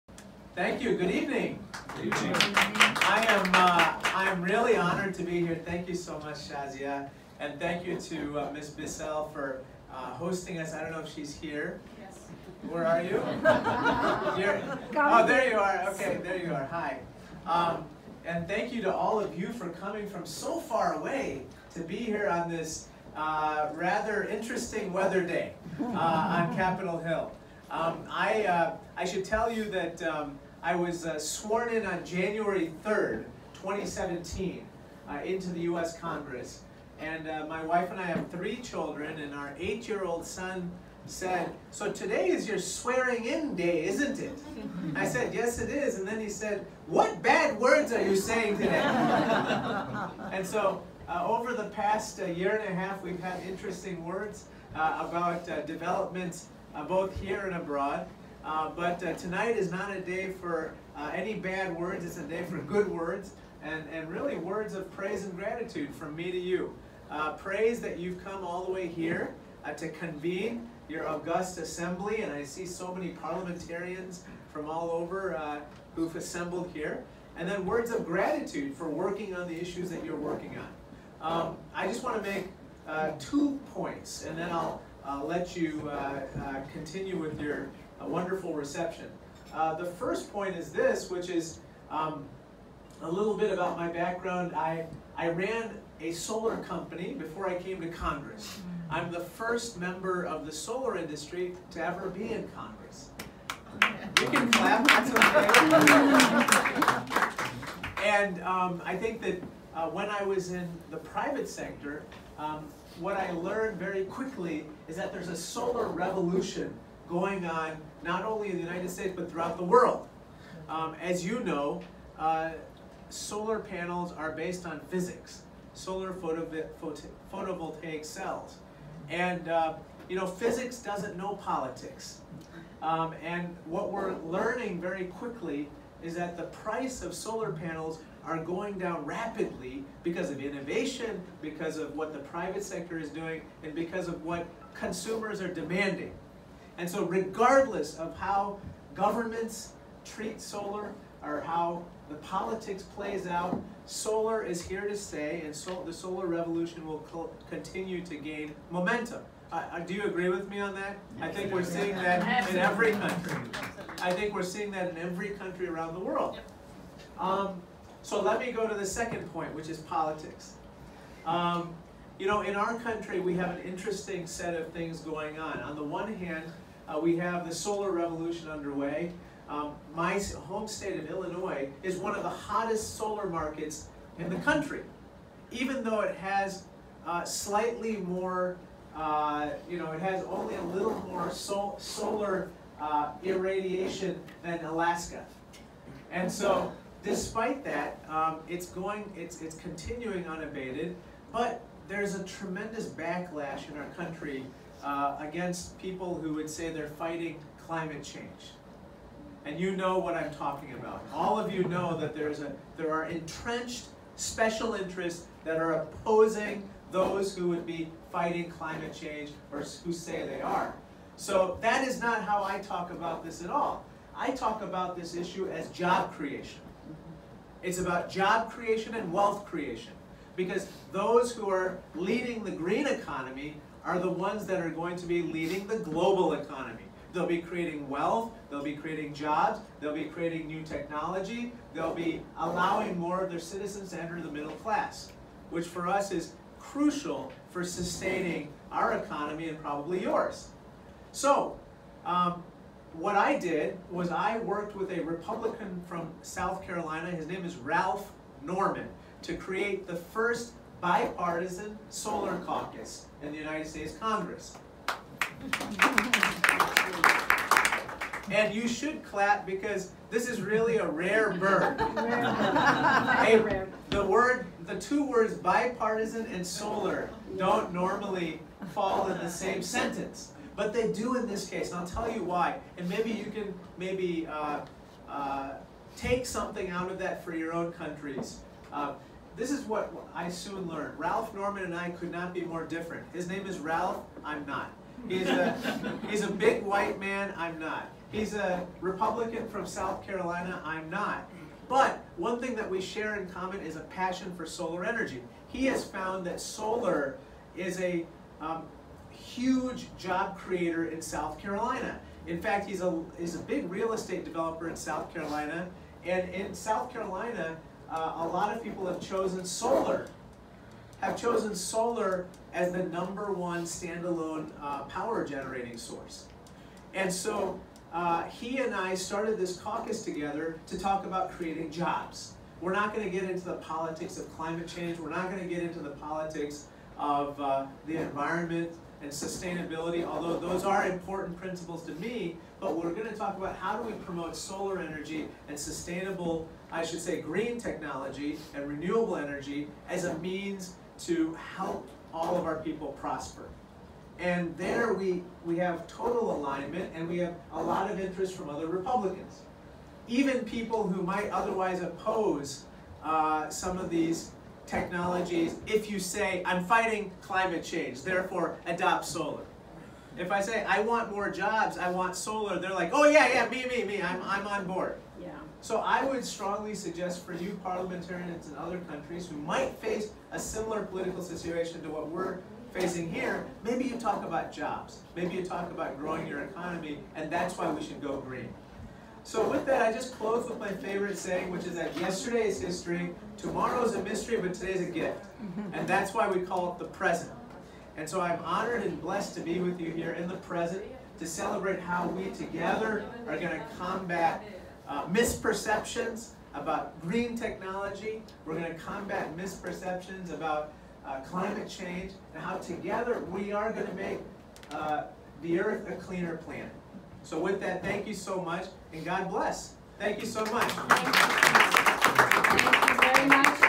Hon. Rep. Raja Krishnamoorthi at AQA HLSS2 Reception
Hon. Rep. Raja Krishnamoorthi (D-IL), U.S. Congress addresses those in attendance at the Second AirQualityAsia High-Level Strategy Session (HLSS2) Reception at the US-Asia Institute on April 17, 2018. Introduction given by Ms. Shazia Z. Rafi, President and Convenor, AirQualityAsia.